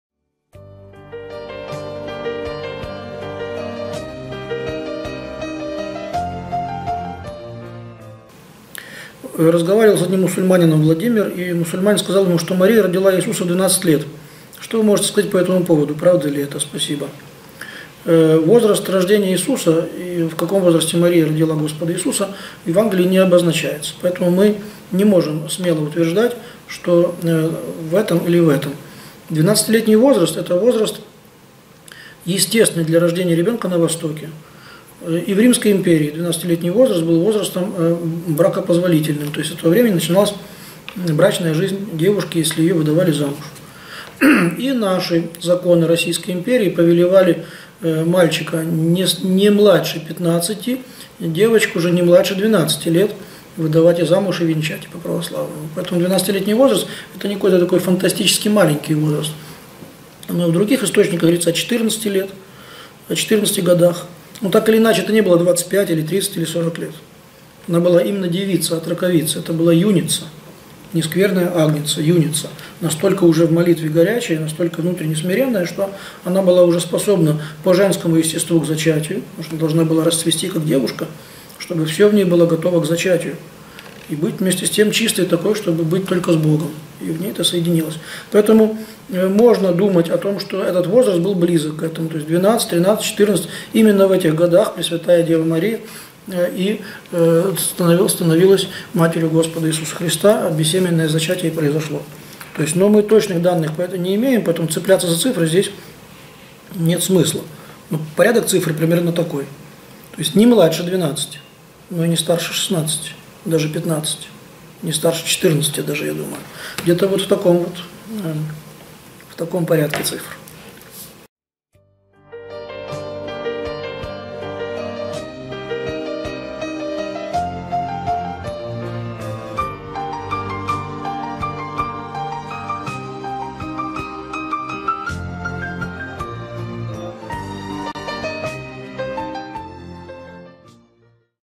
В каком возрасте Богородица родила Христа? Протоиерей Андрей Ткачев - 2017
«Разговаривал с одним мусульманином, и мусульманин сказал, что Мария родила Иисуса в 12 лет. Что вы можете сказать по этому поводу? Правда ли это? Спасибо».
Возраст, в котором Мария родила Иисуса, в Евангелии не обозначается. Мы не можем смело утверждать, что в этом или в этом. 12-летний возраст – это возраст, естественный для рождения ребенка на Востоке и в Римской Империи это был возраст бракопозволительный. С этого времени начиналась брачная жизнь девушки, если ее выдавали замуж. И наши законы, Российской Империи, повелевали мальчика не младше 15-ти, девочку не младше 12-ти лет выдавать замуж и венчать. Поэтому 12-летний возраст – это не какой-то там фантастически маленький возраст. Но в других источниках говорится о 14-ти годах, но так или иначе это не было двадцать пять или тридцать-сорок лет. Она была именно девица, отроковица. Настолько уже в молитве горячая, настолько внутренне смиренная, что Она была уже способна по женскому естеству зачать. Она должна была расцвести как девушка, чтобы все в Ней было готово к зачатию, и быть вместе с тем такой чистой, чтобы быть вместе с Богом. И в Ней это соединилось. Поэтому можно думать, что этот возраст был близок к этому, т.е. 12-13-14 лет, именно в этих года Пресвятая Дева Мария и становилась матерью Господа Иисуса Христа, бессемянное зачатие и произошло. Но мы точных данных не имеем, поэтому цепляться за цифры здесь не имеет смысла. Но порядок цифр приблизительно такой, не младше 12-ти, но и не старше 16-ти. Не старше 14-ти даже, я думаю. Где-то вот в таком порядке цифр.